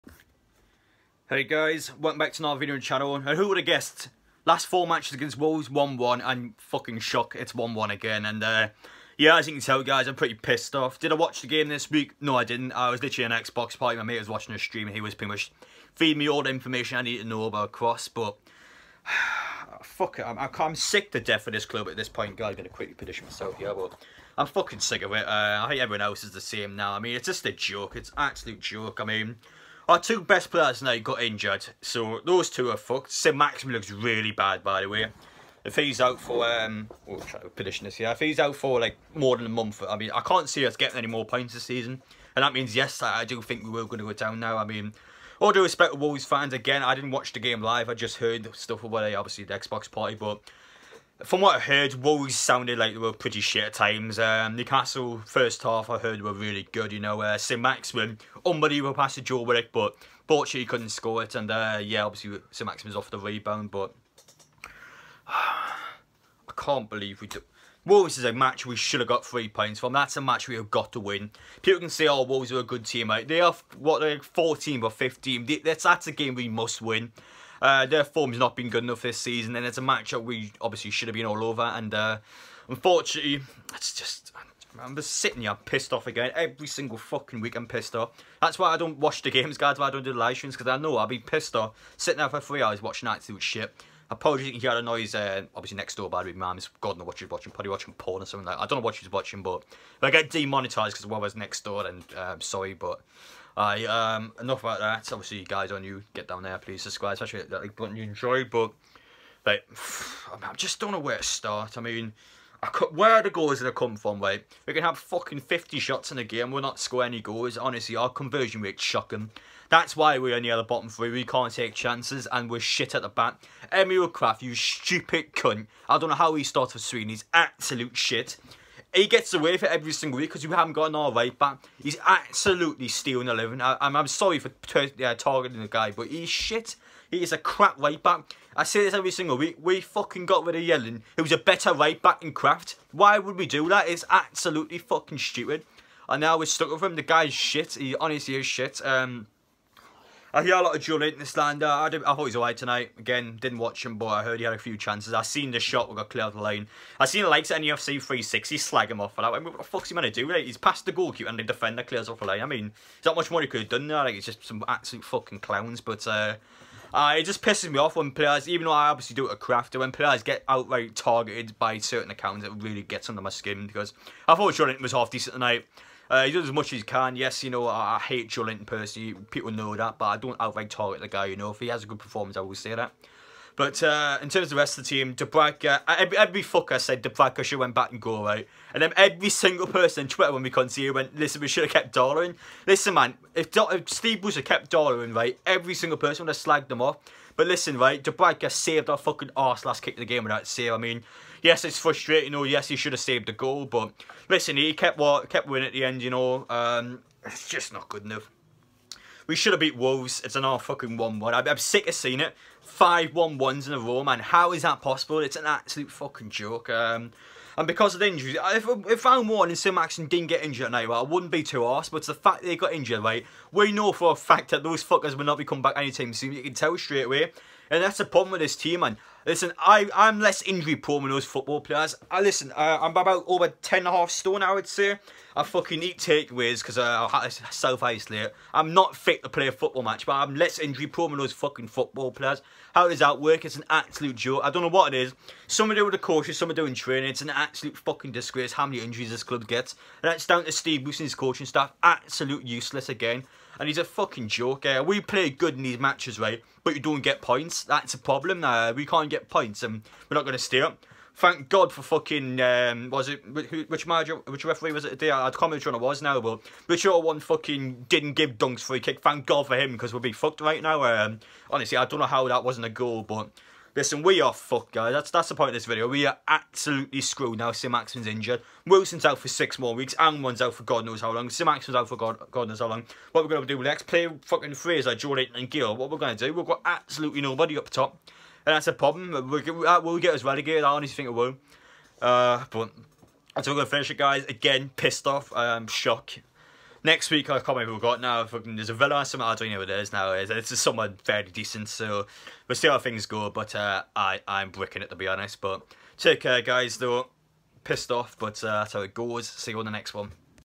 *SAINT-MAXIMIN AND ALMIRON INJURED* NEWCASTLE 1-1 WOLVES MATCH REVIEW!!!!!
Shocking result for Newcastle as they draw 1-1 with Wolves Newcastle Wolves #1-1